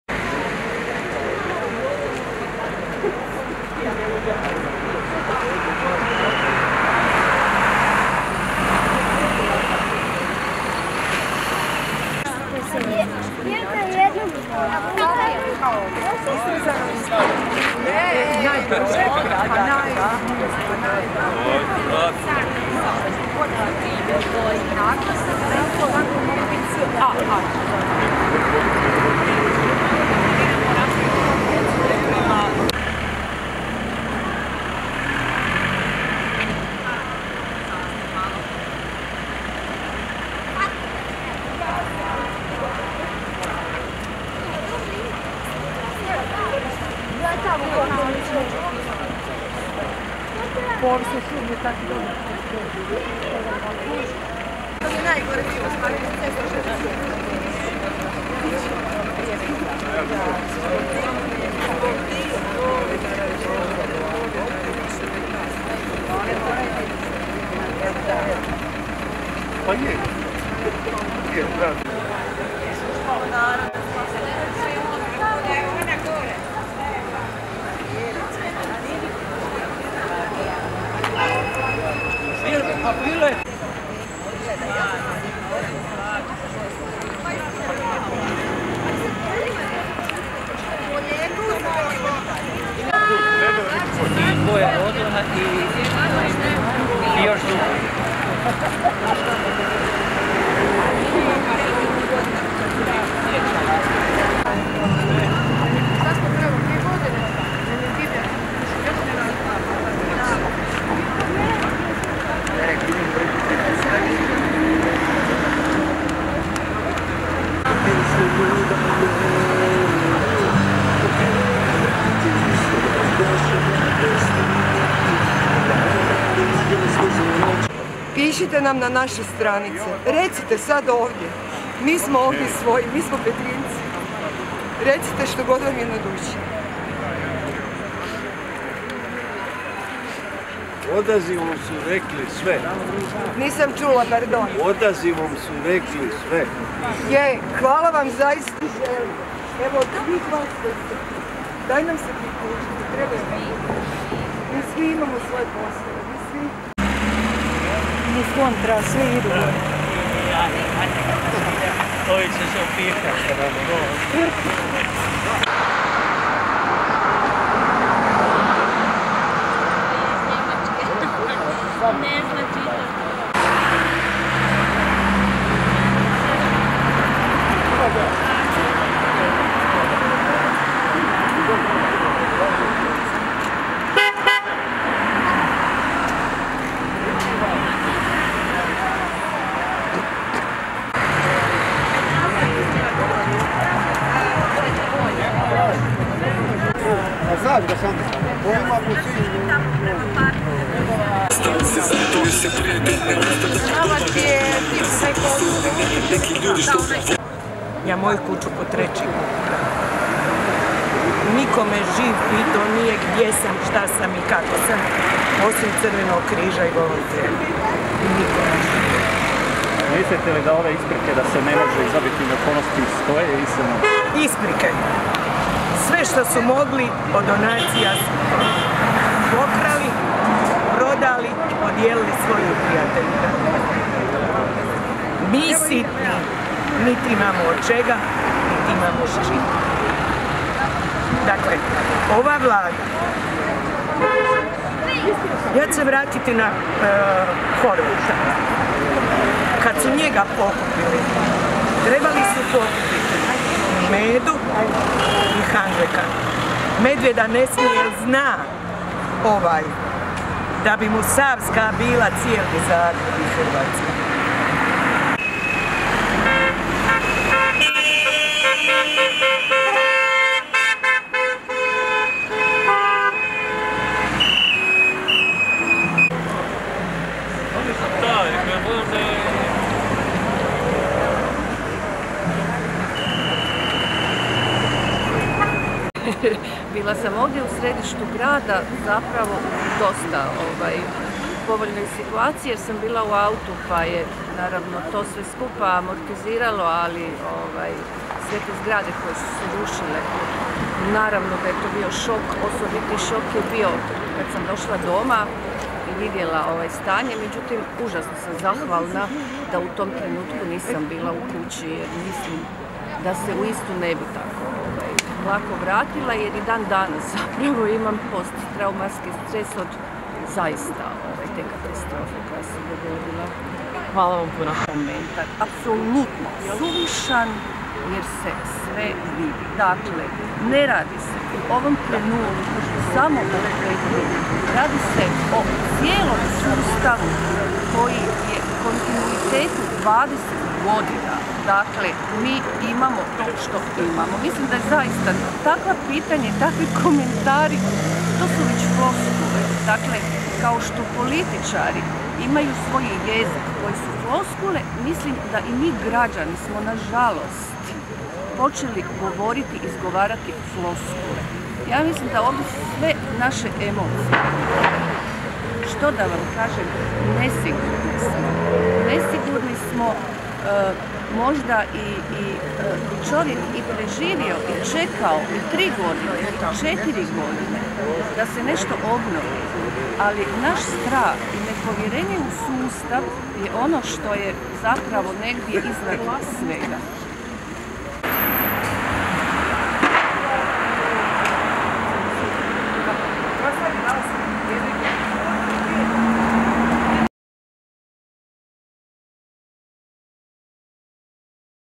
Prosvjed u Petrinji zbog spore obnove
Stanovnici Petrinje prosvjedovali na godišnjicu potresa zbog sporosti vladajućih i obnove. Druga je obljetnica potresa koji je razorio Petrinju i okolicu, a do tada je izgrađeno samo šest zamjenskih kuća. Mnogi stanovnici još uvijek žive u kontejnerima od 25 metara i čekaju povratak svojim domovima.